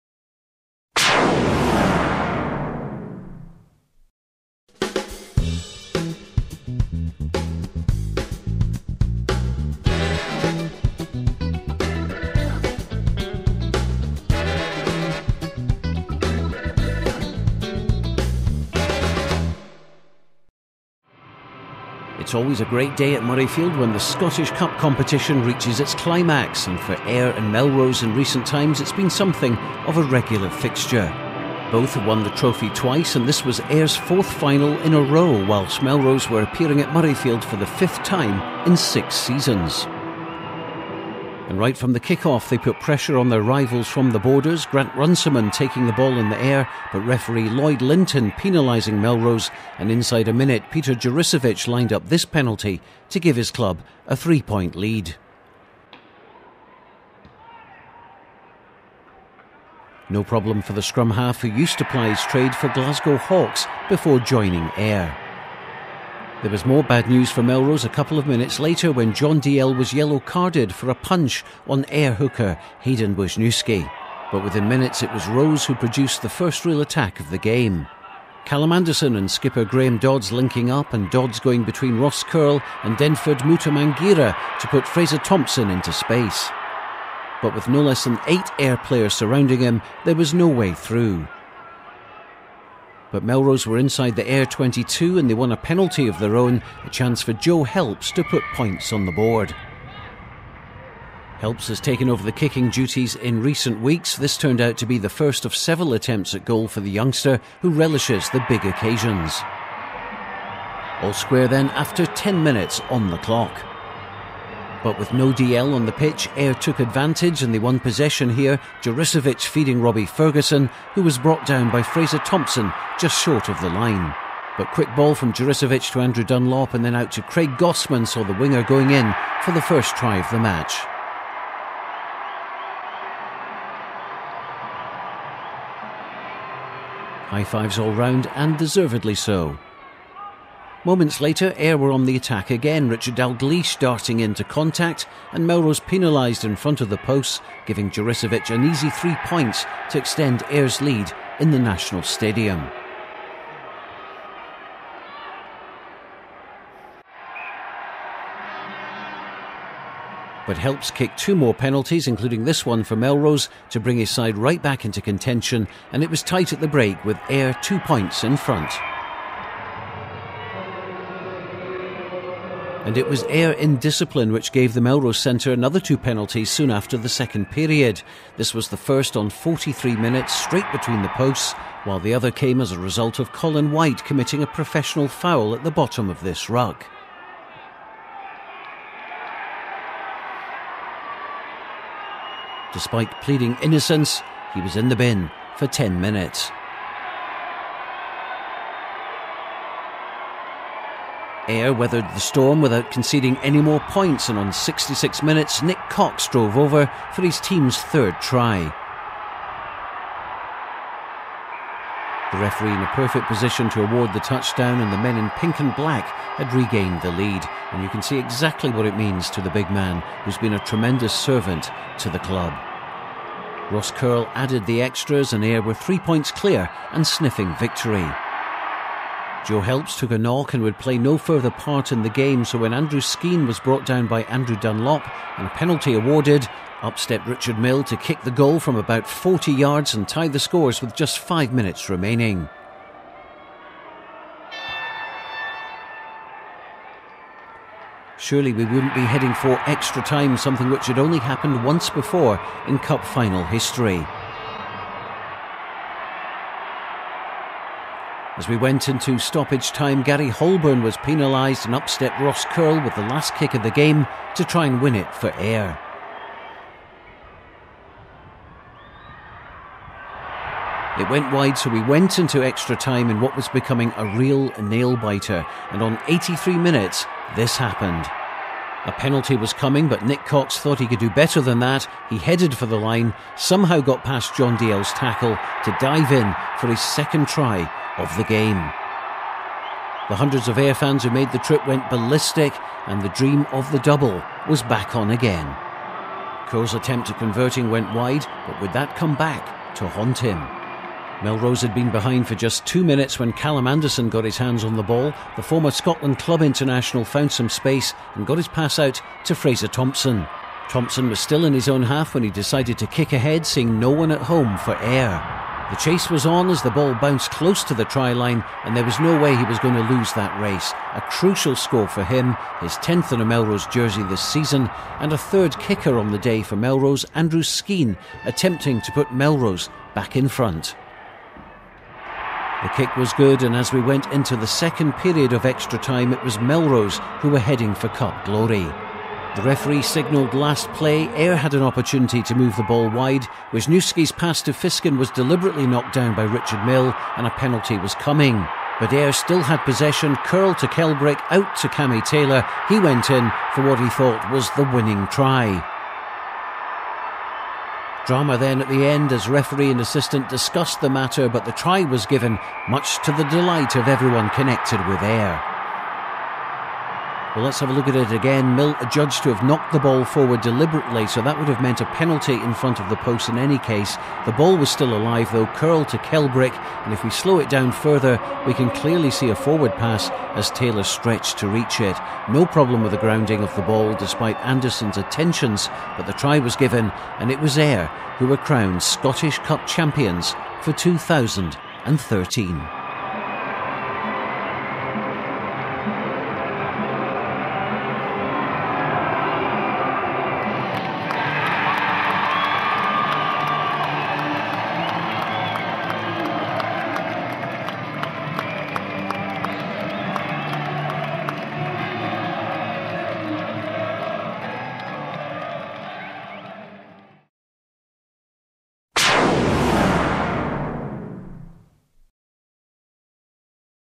2013 SCOTTISH RUGBY CUP FINAL HIGHLIGHTS - MELROSE v AYR
Stuart Cameron reports from Murrayfield on the 2013 Scottish Club Rugby Final between Melrose and Ayr.